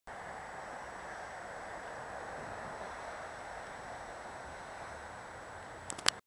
Coach USA/Community Coach: 8768 MCI D4500 on 77
Morristown Bound bus is at Route 495